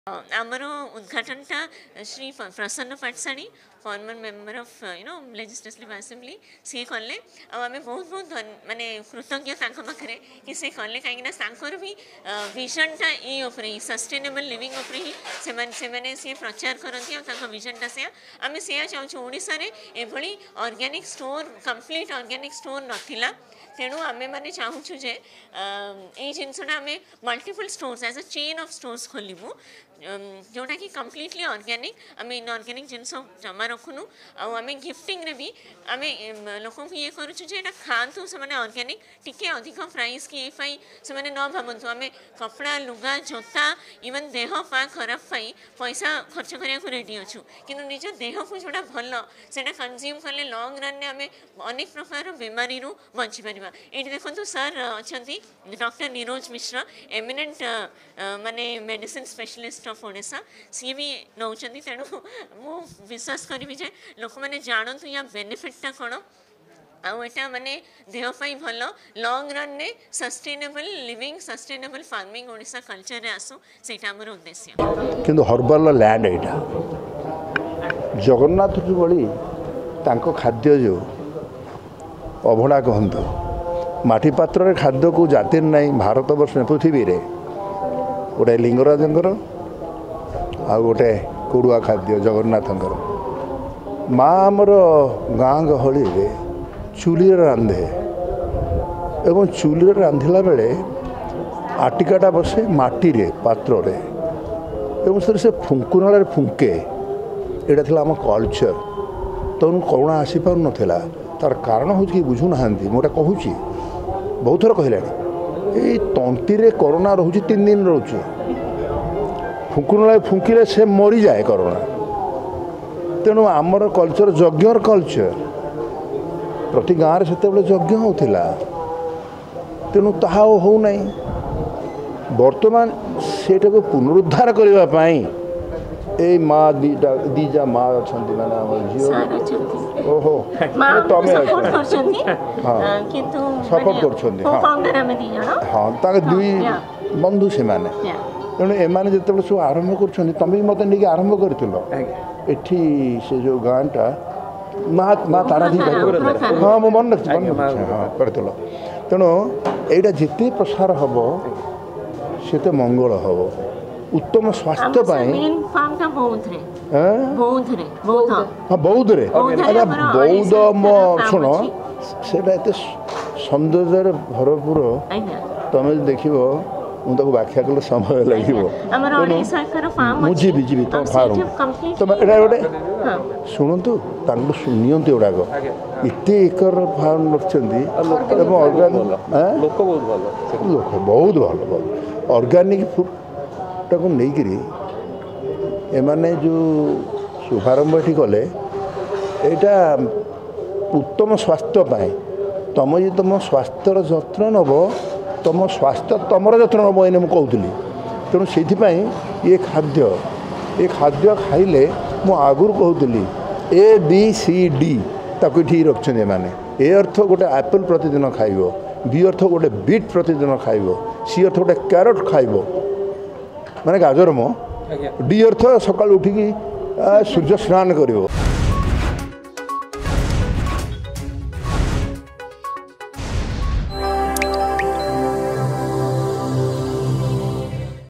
First premium organic food store ‘Get back to Nature’ inaugurated in Bhubaneswar
Bhubaneswar’s one of its kind premium organic food brand  ‘Get Back to Nature' inaugurated in Cosmopolis Apartments, Dumduma.